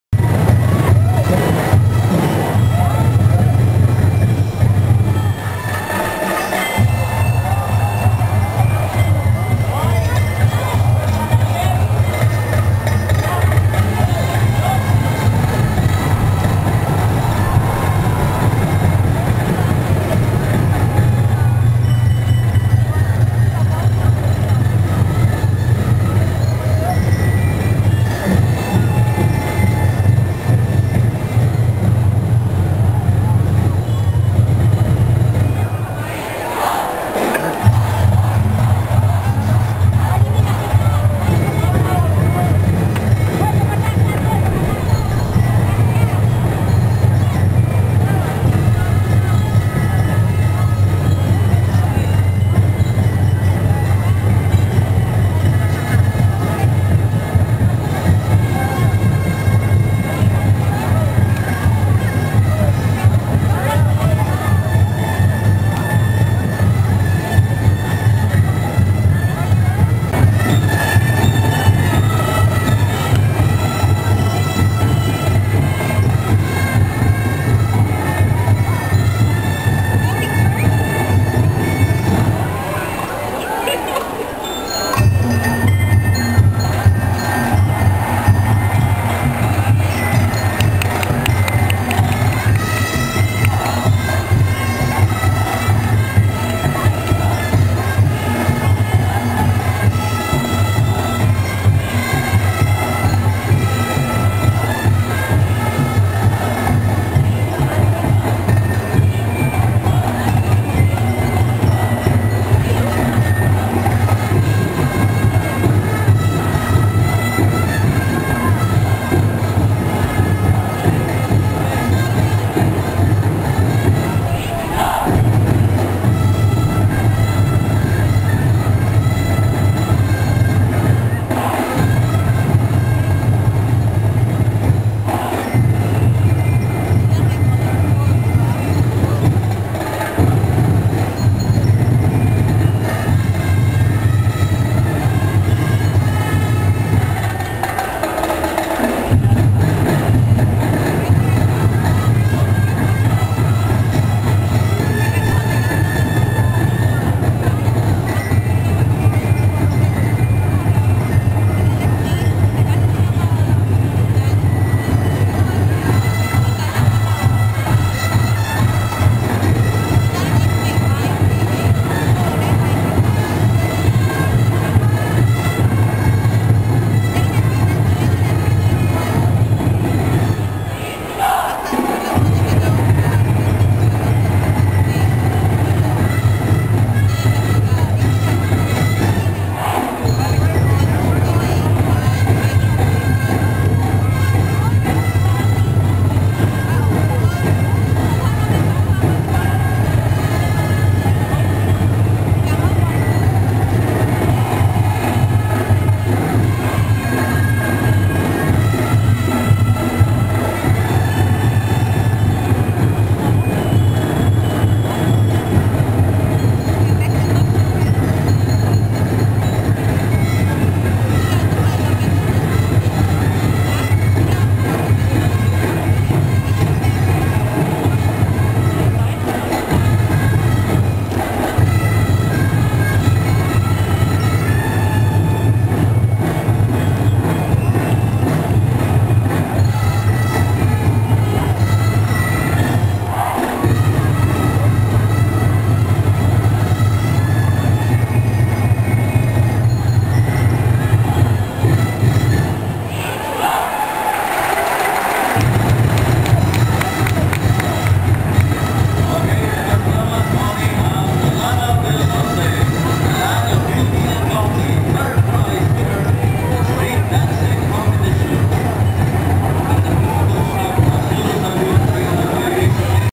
Sinulog 2010 Street Dance - Lanao Del Norte
Repeat performance of the winning contingents during the awarding ceremonies. More of the stories at my blog.